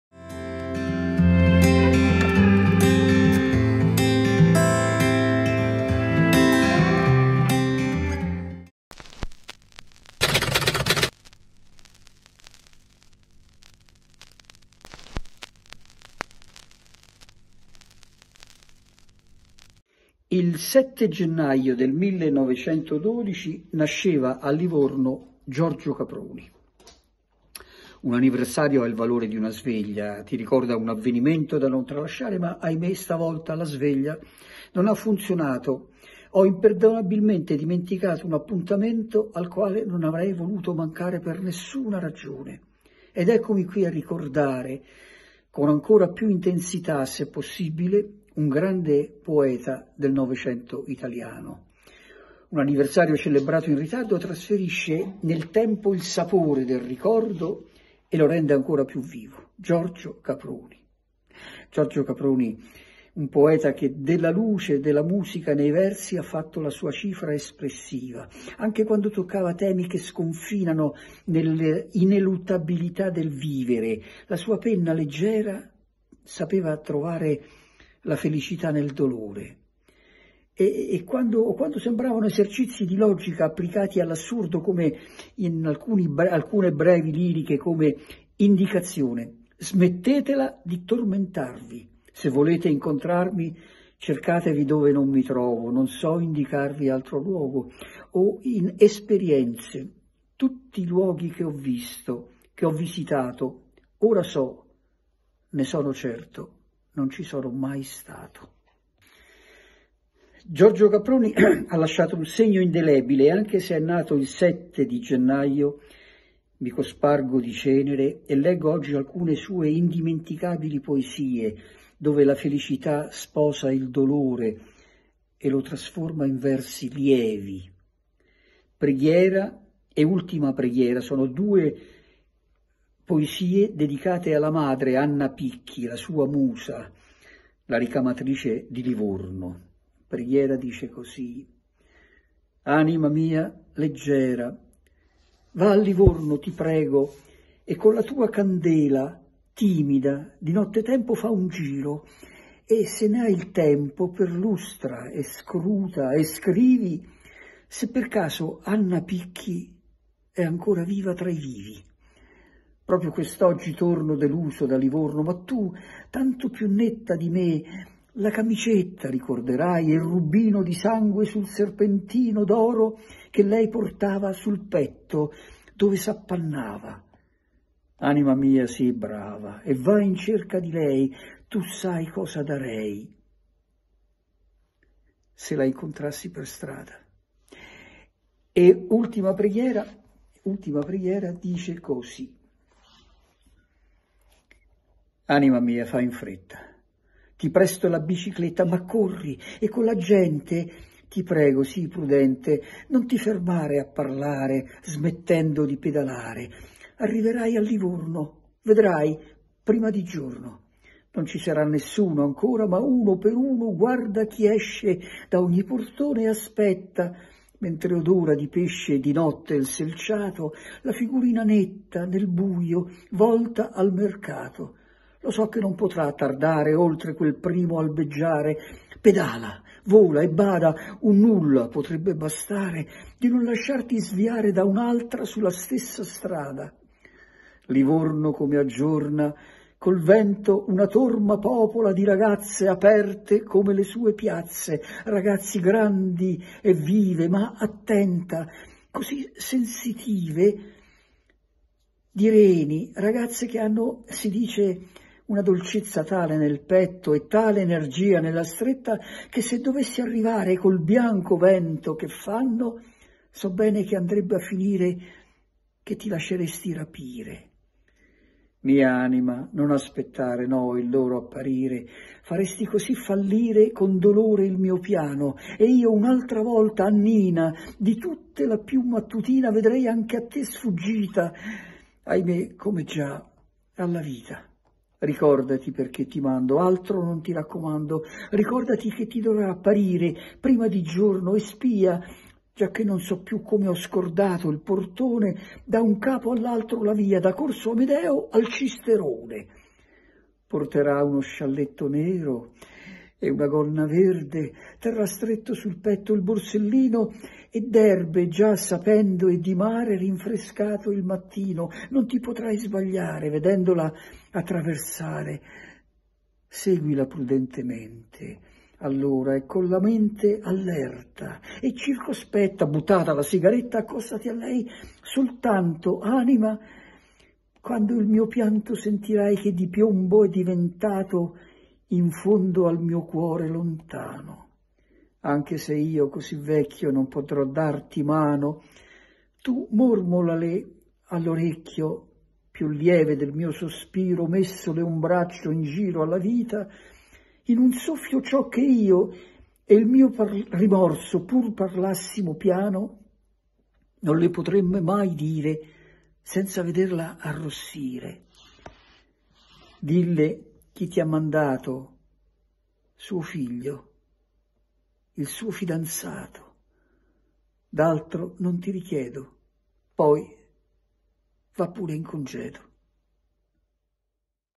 GIORGIO CAPRONI
A Livorno c'è una targa a lui dedicata che dice: "Qui nacque Giorgio Caproni, poeta delicato e forte come la città che lo vide nascere".
Lo scrittore Angelo Zito ce lo ricorda con questo suo breve intervento.